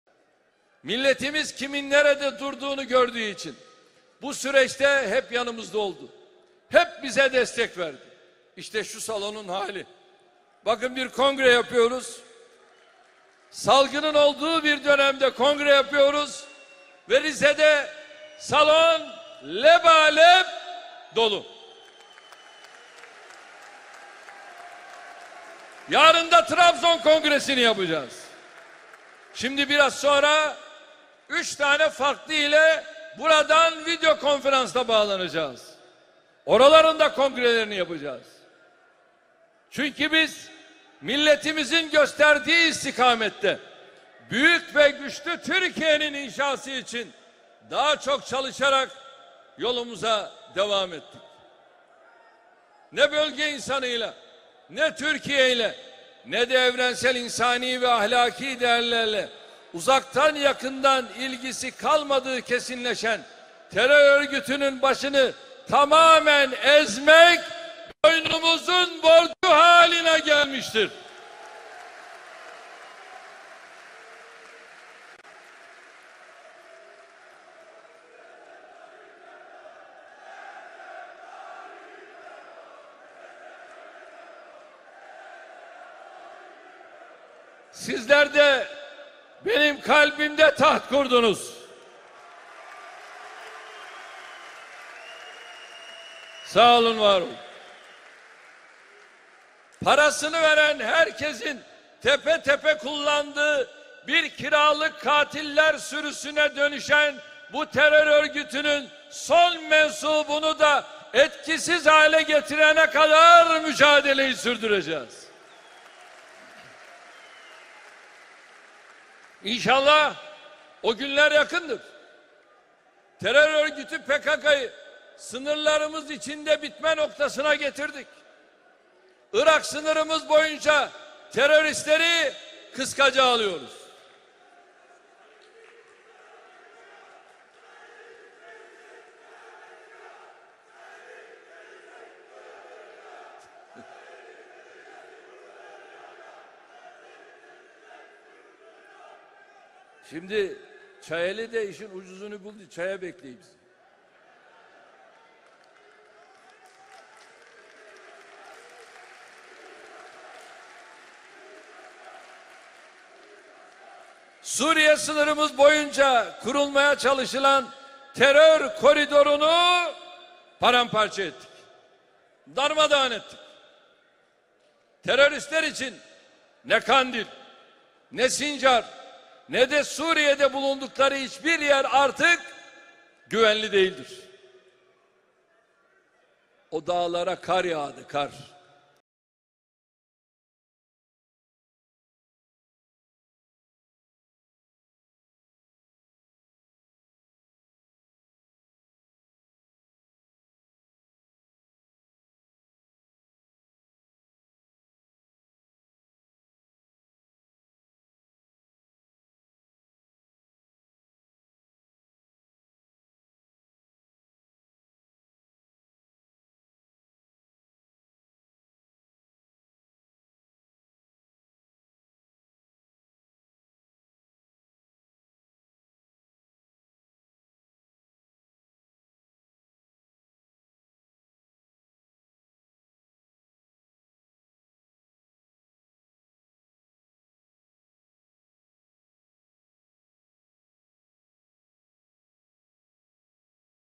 13 şehidin ardından Erdoğan'ın bu görüntüsü olay oldu
Cumhurbaşkanı Erdoğan'ın 13 şehidin ardından 1 gün sonra düzenlenen Rize'deki kongresi tepkilere neden oldu. Erdoğan'ın kongredeki gülüşü ise sosyal medyada "Daha 36 şehit varken gülen şahıs, 13 şehidimiz varken yine gülüyor" tepkisi ile karşılaştı. Siyasiler ve birçok yurttaş bu duruma isyan etti.